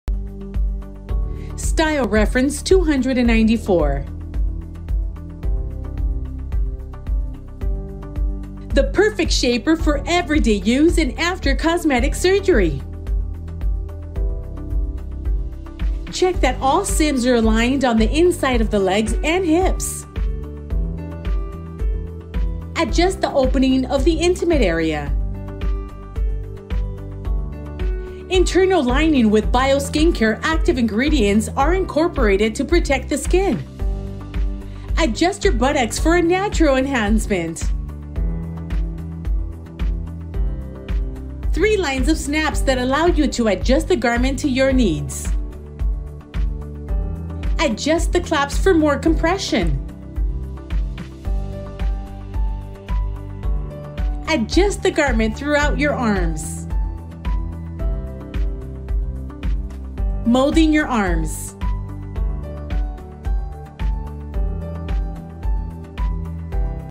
💖Shape and Lift with CYSM Firm Control Arm Shapewear Girdle with Butt Lifting
Discover the CYSM Firm Control Arm Shapewear Girdle with Butt Lifting, the perfect solution for enhancing your curves and providing firm control. In this detailed review, we explore how this innovative shapewear smooths your silhouette, lifts your butt, and offers targeted support for your arms. Ideal for everyday wear, it ensures you look and feel your best all day long. Watch now to learn why CYSM is a must-have in your shapewear collection!